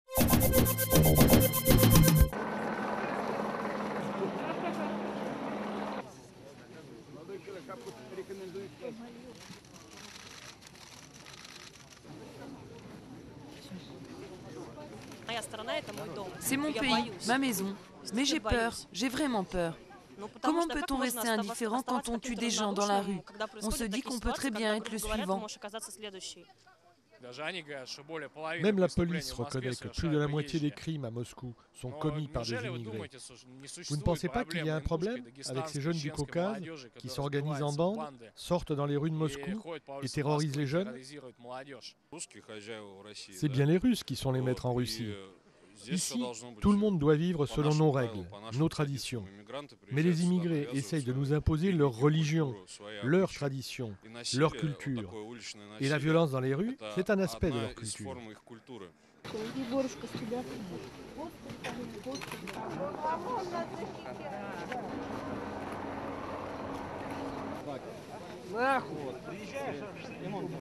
Manifestation nationaliste à Moscou
Entre 200 et 300 supporters et ultra-nationalistes se sont rassemblés samedi 15 janvier à Moscou, en mémoire de l'un des leurs, tués en décembre, lors d'une rixe avec des Caucasiens. Cette mort avait déclenché une série de manifestations et de violences nationalistes. Les manifestants se sont rendu à l'abri bus où le jeune supporter a trouvé la mort. Ils y ont déposé des fleurs et des bougies. Les leaders d'extrême droites ont pris part au rassemblement.  Durée:01:10